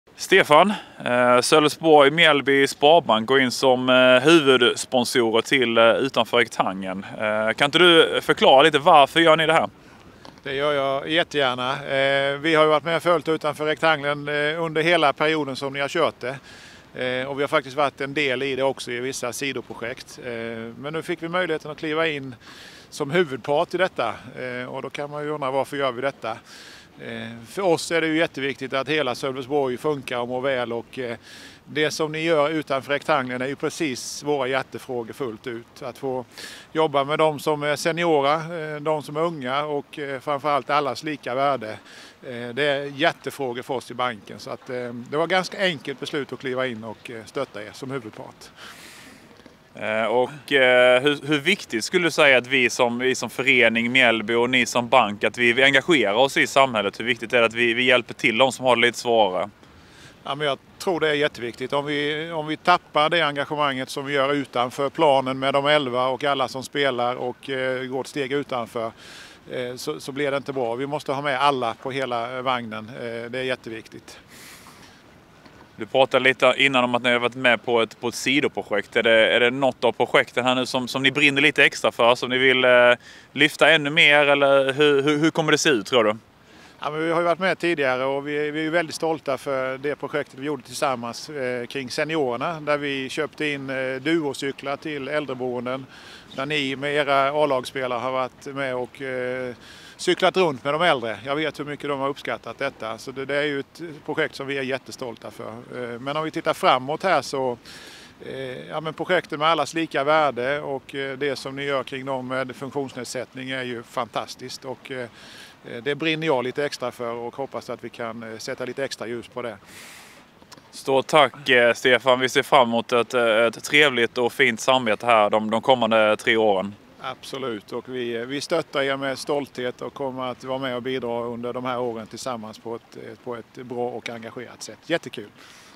Intervju med Stefan Ohlson
Intervju med Sölvesborg-Mjällby Sparbanks VD Stefan Ohlson som pratar om att bli huvudpartner för Utanför Rektangeln.